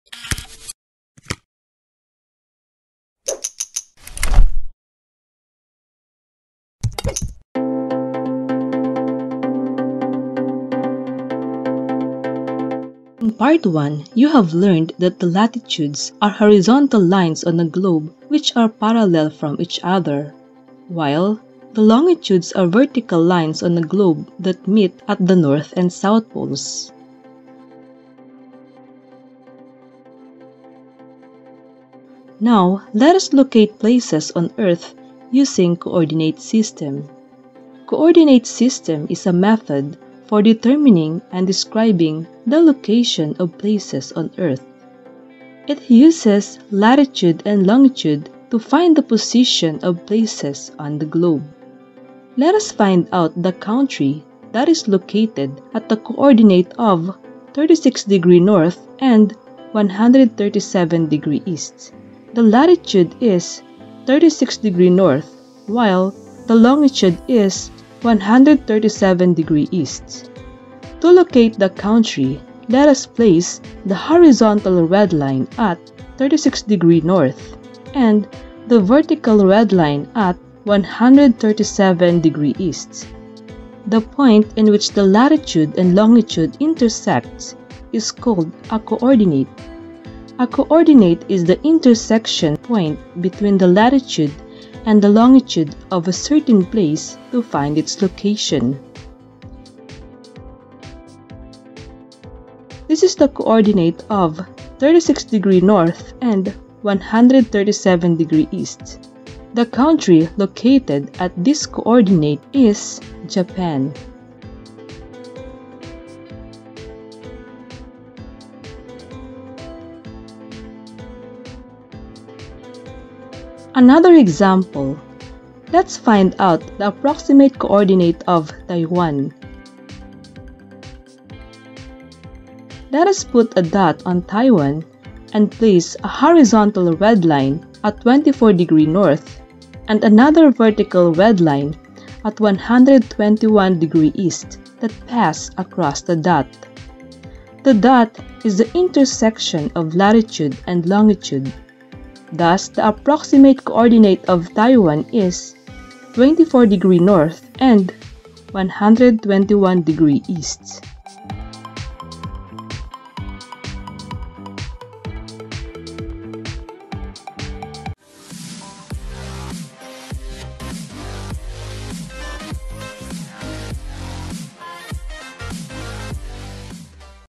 How to locate places on earth using latitude and longitude/ Coordinate System? I PART 2
AWESOME SCIENCE EXPERIMENTS

Locating Places on Earth Using Coordinate System

Coordinate System is a method for determining and describing the location of places on earth. It uses latitude and longitude to find the position of places on the globe.

Coordinate is the intersection point between the latitude and the longitude of a certain place to find its location.

How to locate places on earth using latitude and longitude?
How to locate places on earth using Coordinate System?